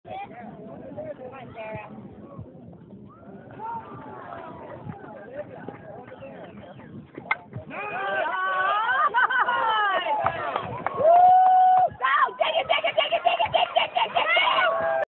Slamming sarah
My granddaughter hitting some slammers at her softball game